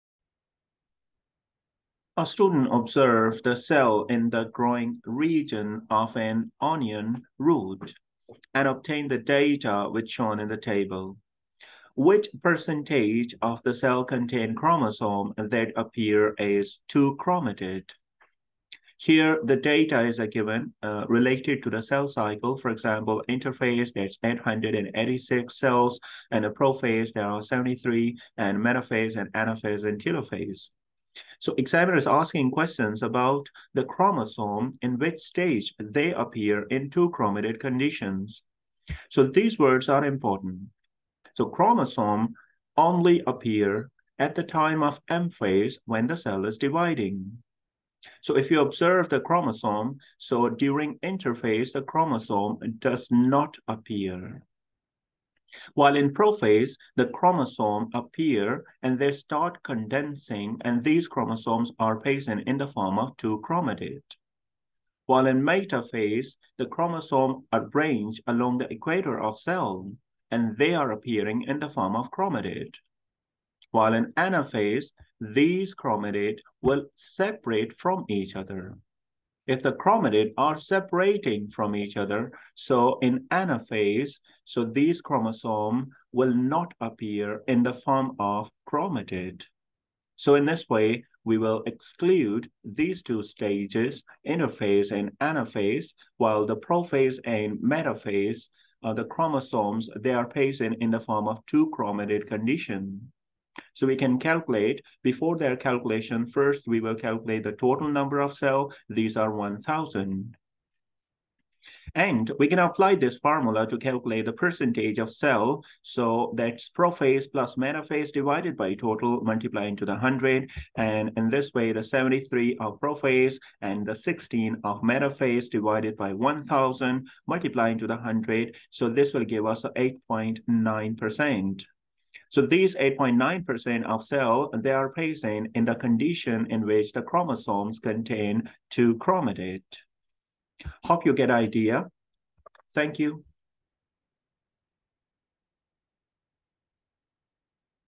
Calculate percentage of cells contain chromosomes that appear as two chromatids
basic science concepts in education like practical exams’ solving strategies, critical, theoretical, analytical & calculation-based questions.
A student observed the cells in the growing region (meristem) of an onion root and obtained the data shown.
Which percentage of cells contains chromosomes that appear as two chromatids?
Prophase, metaphase, anaphase, telophase, interphase, cell cycle, AS questions